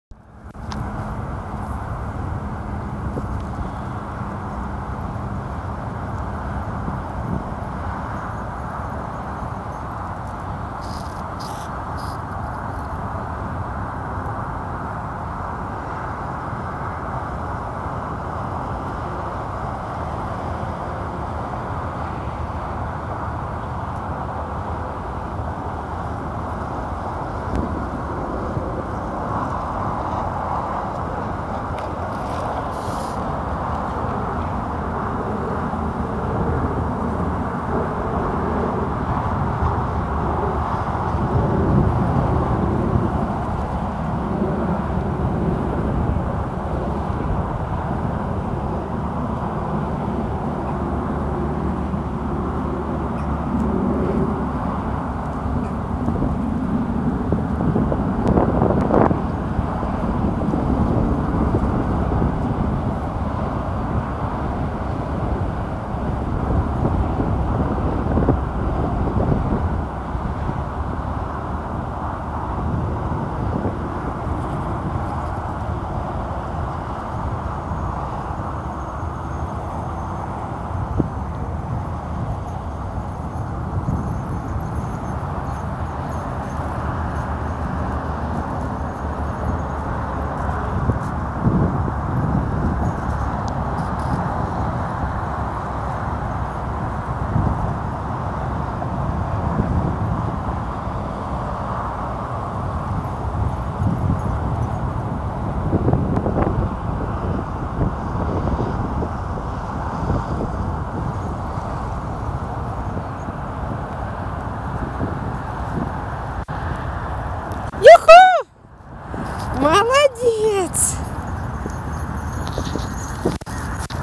Poodle, field training 04apr2019
First field training  this year. Are you see black on the photo? It is burned grass. Unfortunately, we have a lot of idiots across the country, who burn grass. It is a very bad custom, and it is monstrously ignorance. The law forbid it, but...